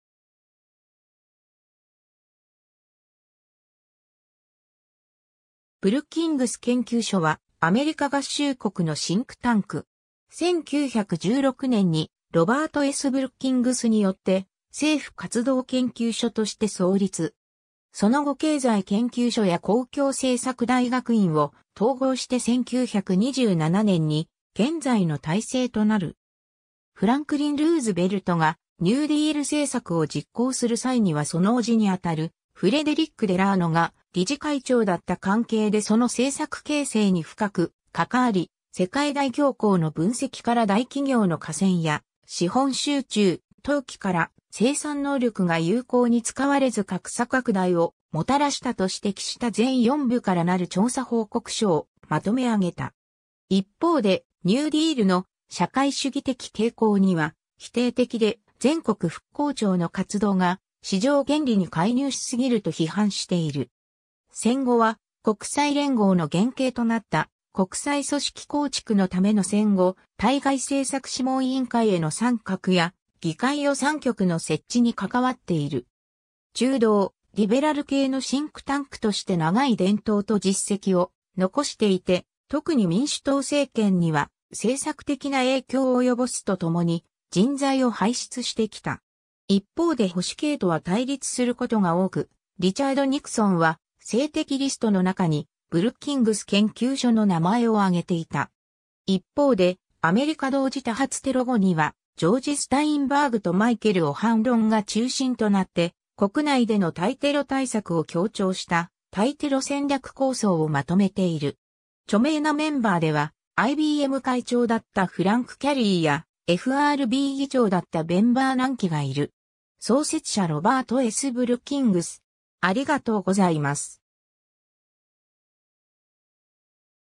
ブルッキングス研究所
ブルッキングス研究所（ブルッキングスけんきゅうじょ、The Brookings Institution）は、アメリカ合衆国のシンクタンク。
 1916年にロバート・S・ブルッキングスによって「政府活動研究所」として創立。
その後経済研究所や公共政策大学院を統合して1927年に現在の体制となる。
 フランクリン・ルーズベルトがニューディール政策を実行する際にはその叔父にあたるフレデリック・デラーノが理事会長だった関係でその政策形成に深く関わり、世界大恐慌の分析から大企業の寡占や資本集中・投機から生産能力が有効に使われず格差拡大をもたらしたと指摘した全四部から成る調査報告書（Capacity Studies）をまとめ上げた。
一方でニューディールの「社会主義的」傾向には否定的で、全国復興庁の活動が市場原理に介入し過ぎると批判している。
戦後は、国際連合の原型となった国際組織構築のための戦後対外政策諮問委員会への参画や、議会予算局の設置に関わっている。
 中道・リベラル系のシンクタンクとして長い伝統と実績を残していて、特に民主党政権には政策的な影響を及ぼすと共に、人材を輩出してきた。
一方で保守系とは対立することが多く、リチャード・ニクソンは政敵リスト（より正確には政敵マスター・リスト）の中にブルッキングス研究所の名前を挙げていた。
一方でアメリカ同時多発テロ後には、ジョージ・スタインバーグとマイケル・オハンロンが中心となって国内での対テロ対策を強調した対テロ戦略構想をまとめている。
 著名なメンバーでは、IBM会長だったフランク・キャリーやFRB議長だったベン・バーナンキがいる。
 創設者　ロバート・S・ブルッキングス